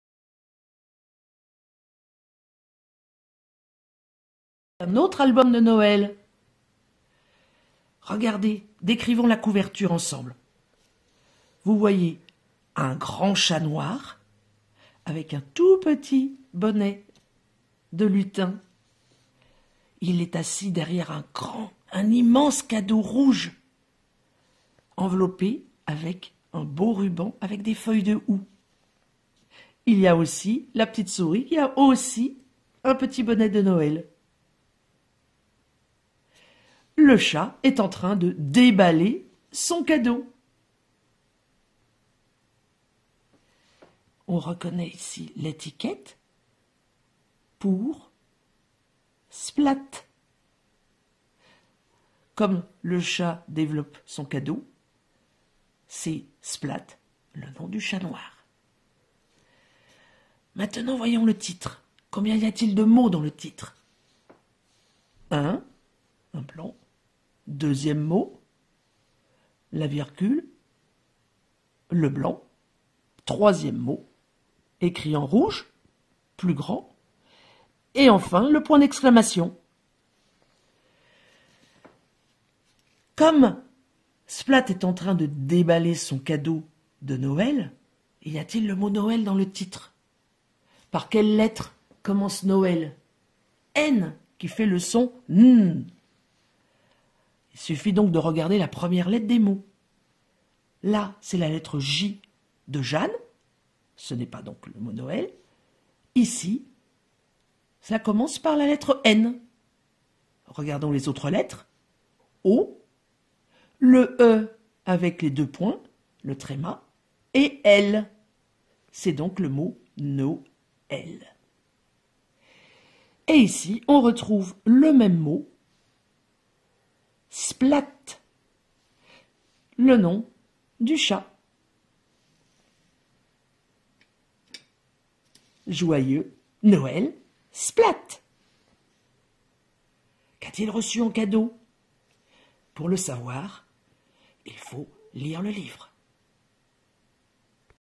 {Analyse de couverture} Joyeux Noël, Splat !
Découverte de couverture d'album :
description illustration, prise d'indices pour lecture du titre
mots nouveaux de vocabulaire